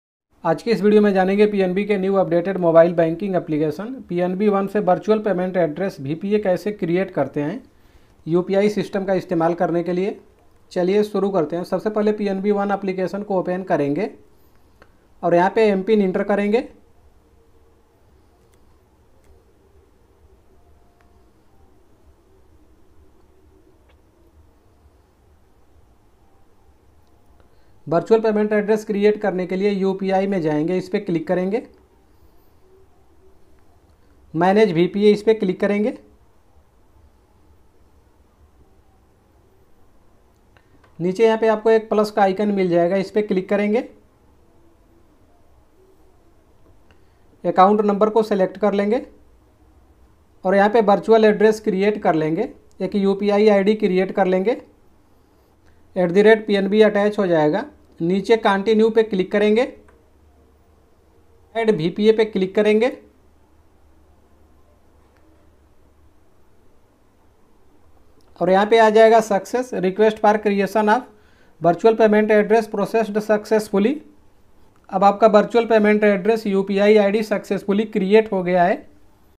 पंजाब नैशनल बैंक मोबाइल बैंकिंग UPI id कैसे Create करें | PNB one Create VPA virtual payment address
Punjab National Bank ke mobile banking application PNB One se virtual payment adress VPN Kaise create Karte Hai video mein complete process ko bataya gaya hai

Dosto aap Ko Bata De upi ID ko virtual payment address VPA bhi bolate Hain